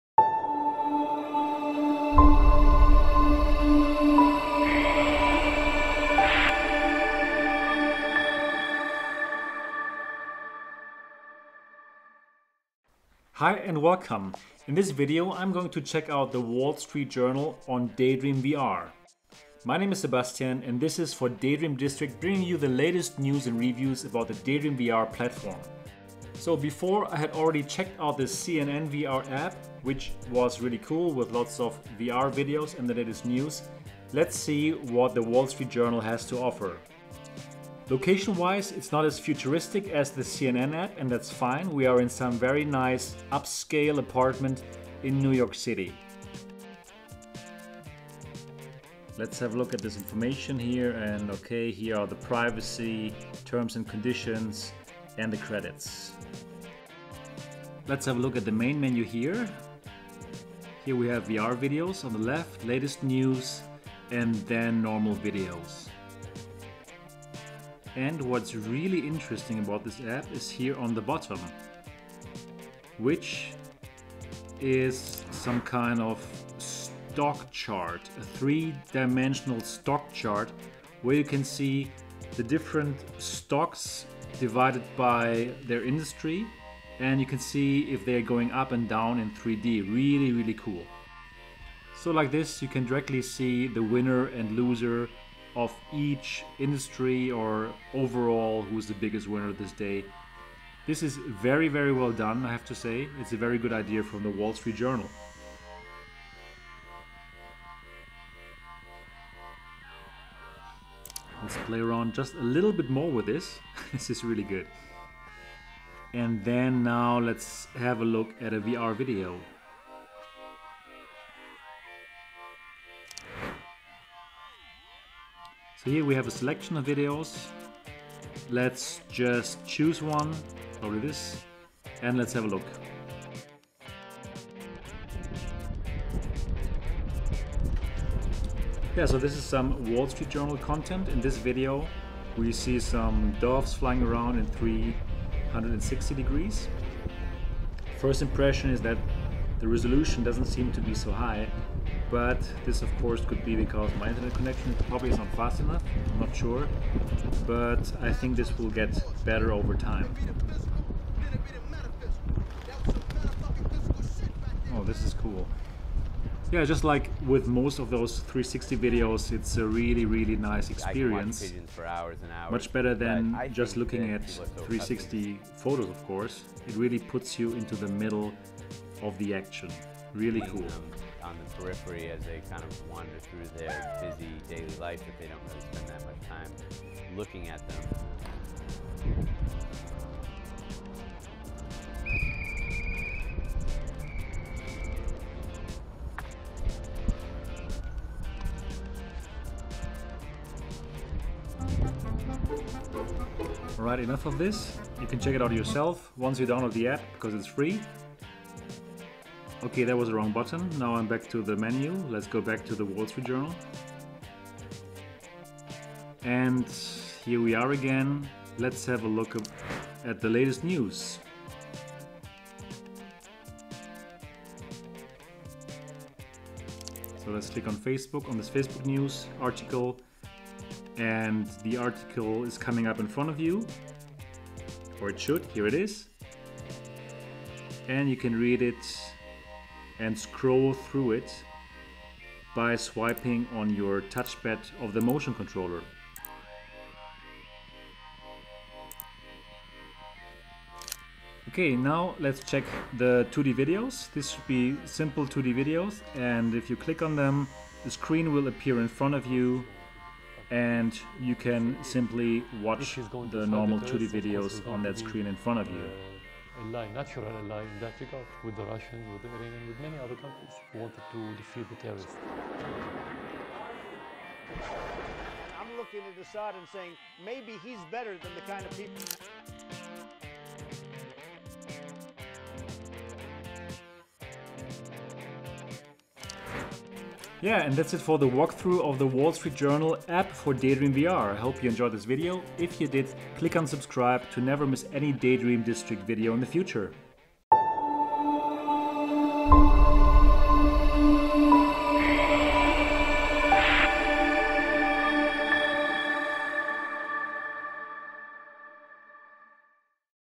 Google Daydream VR: Wall Street Journal Walkthrough / Hands-On
In this video I walk you through the Wall Street Journal App, which is one of the Daydream VR launch titles!

For more on Daydream, simply visit daydreamdistrict.com !